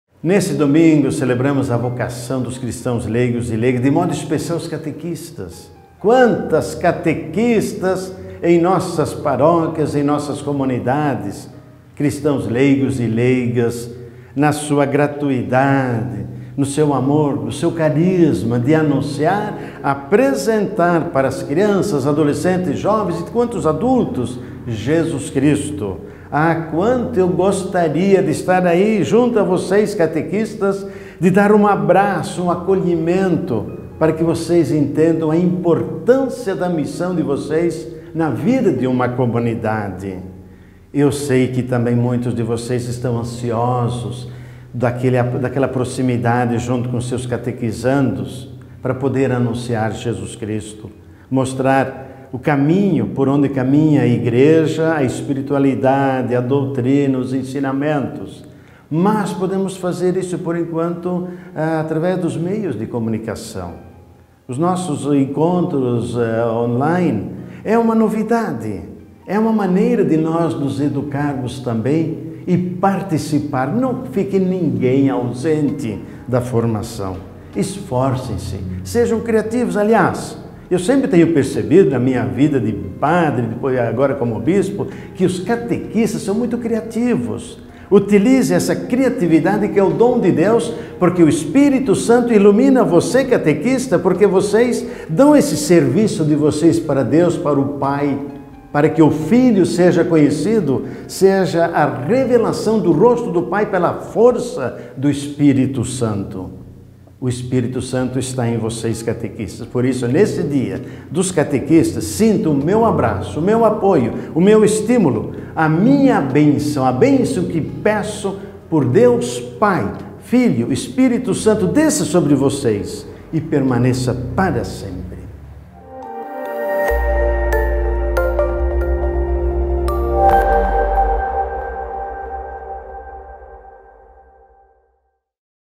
Dia do Catequista 2020 - Mensagem de Dom Severino Clasen
Mensagem do Arcebispo de Maringá, Dom Severino Clasen, por ocasião do Dia do Catequista 2020, celebrado domingo, 30 de agosto.